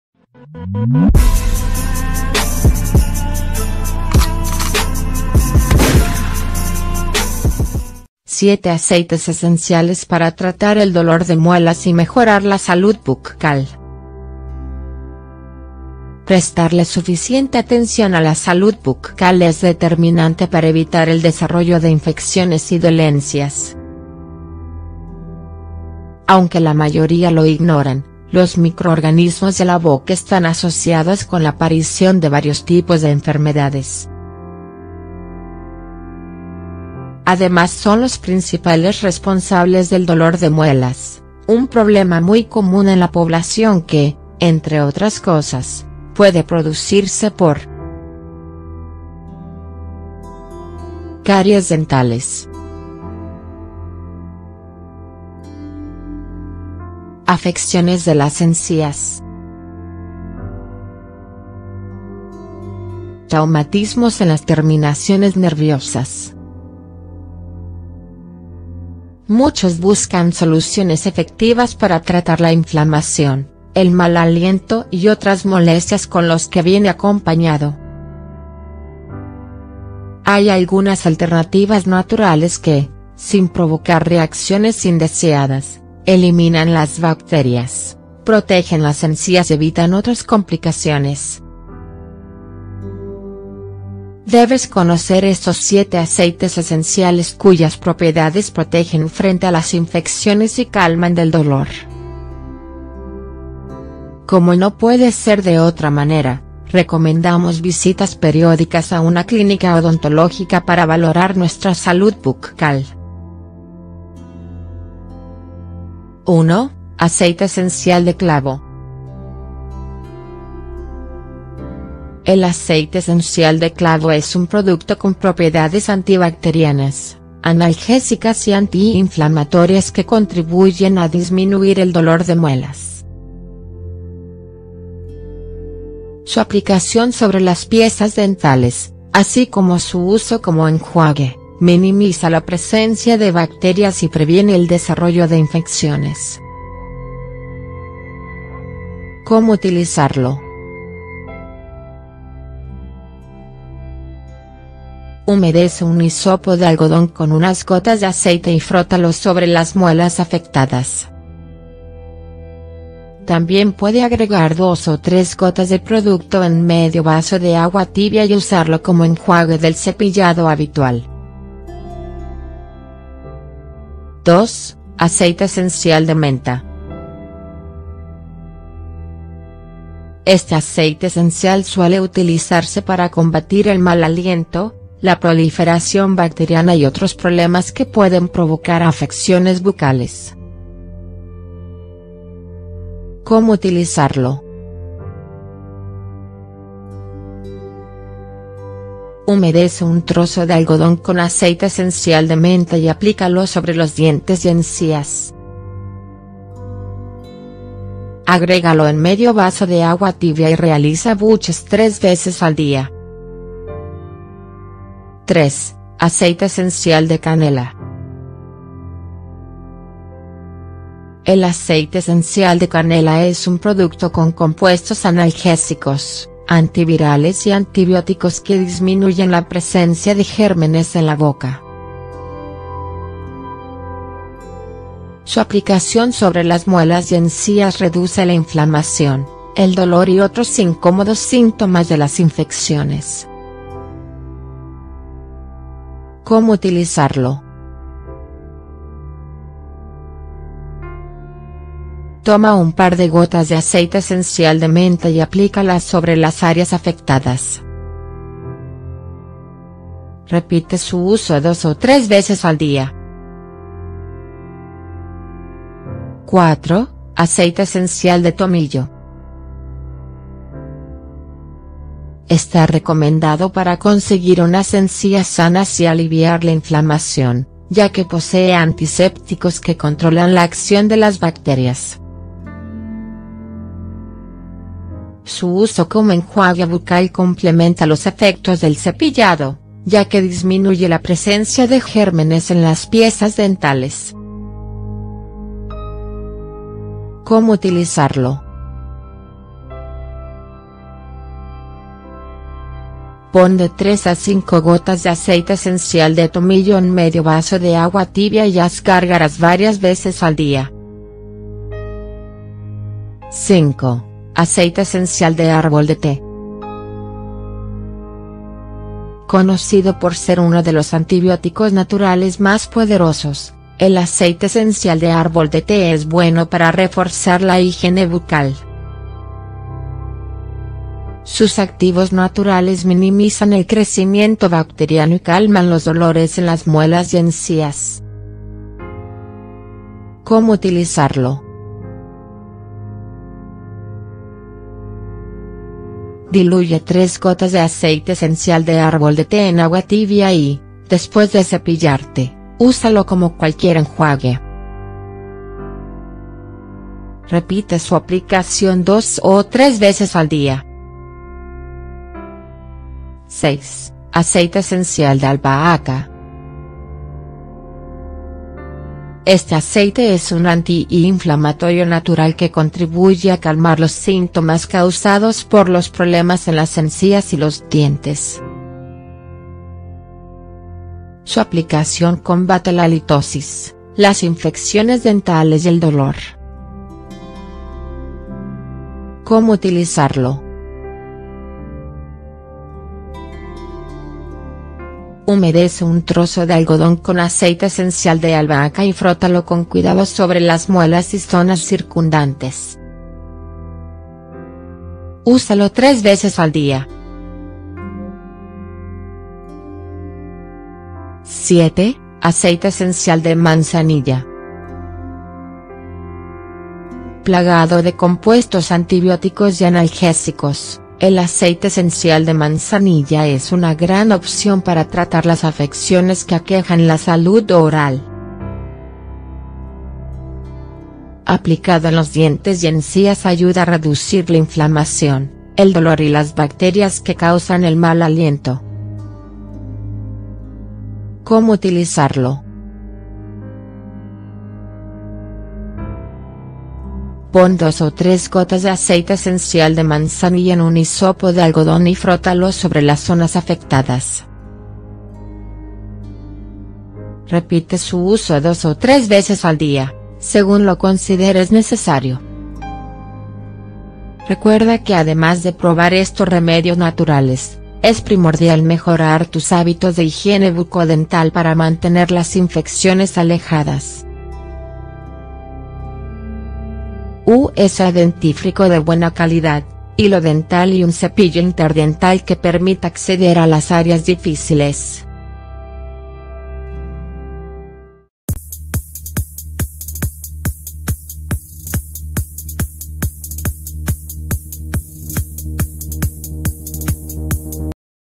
7 aceites esenciales para tratar el dolor de muelas y mejorar la salud bucal
Las propiedades de algunos aceites esenciales son útiles para tratar el dolor de muelas y otros problemas que aquejan la salud bucal. ¡Descúbrelos!